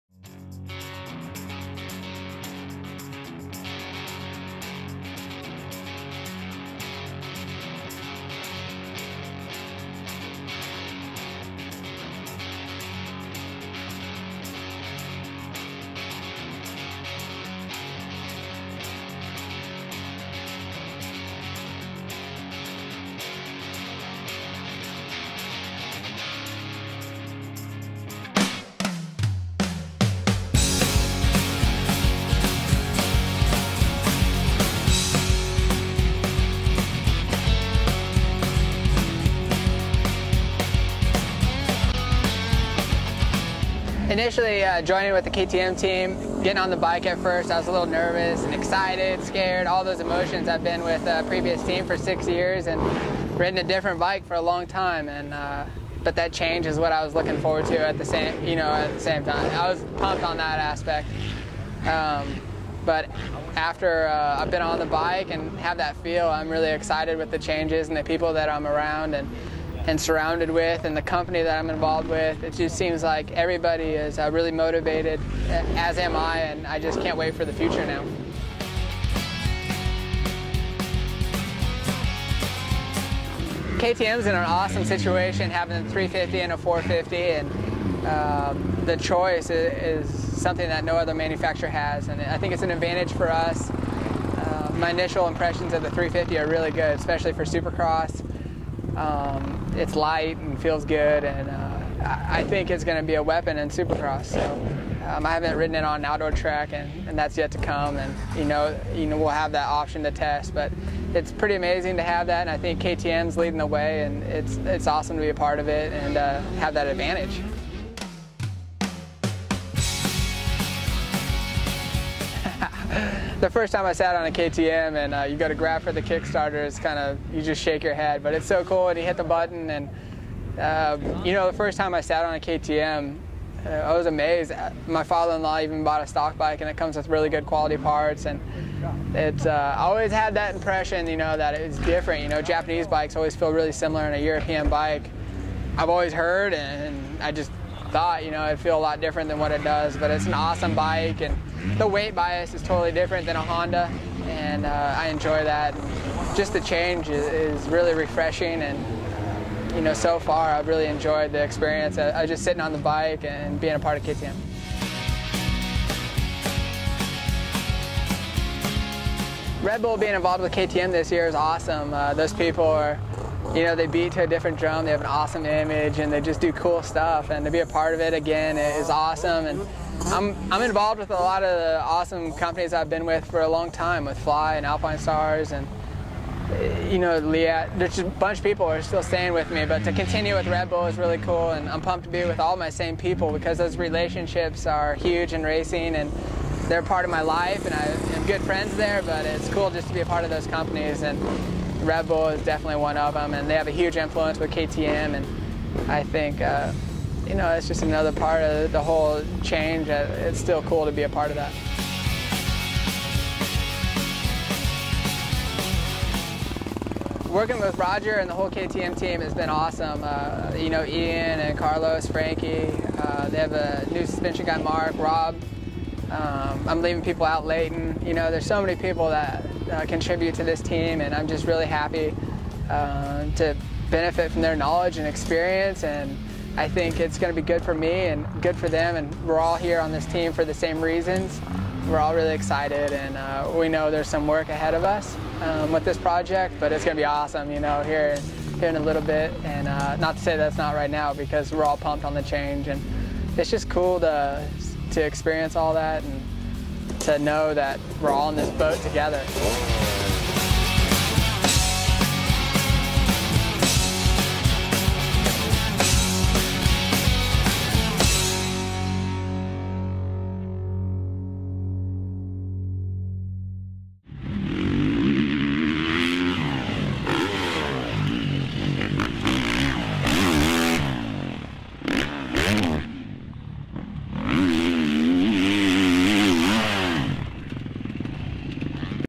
Andrew Short on his Red Bull KTM
Andrew Short out riding on his new Red Bull KTM. Film by Steve Cox.